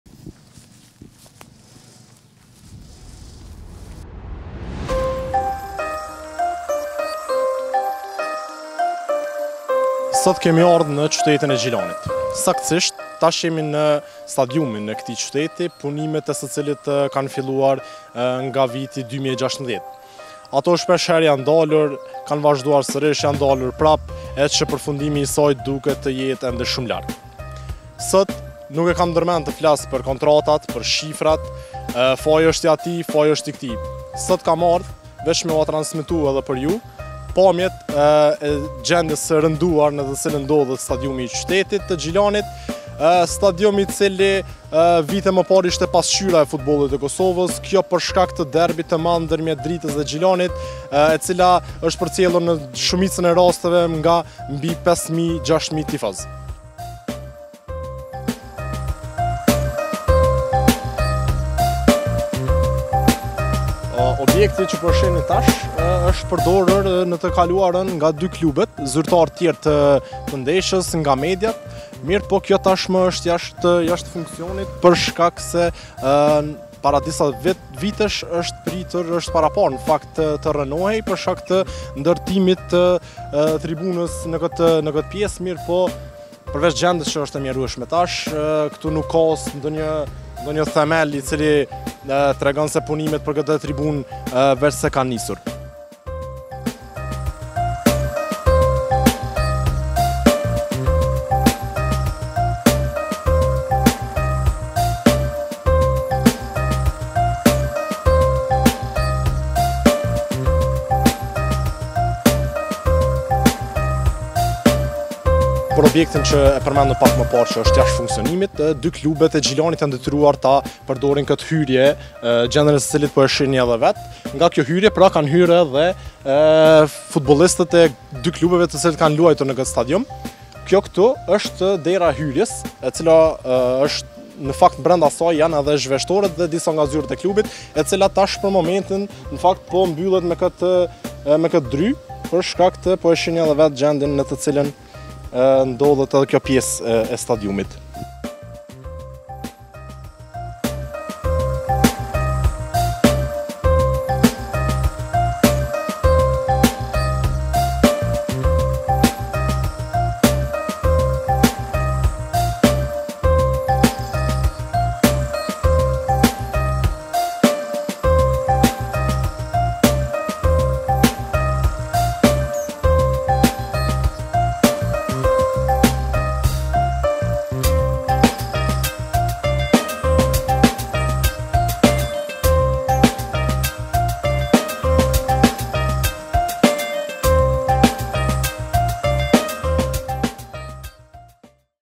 Stadiumi i Gjilanit si mos më keq, në gjendje të rëndë
Stadiumi i Gjilanit si mos më keq, impianti që ishte “pasqyrë” e futbollit të Kosovës në gjendje të rëndë, gazetari tregon se përfundimi është shumë larg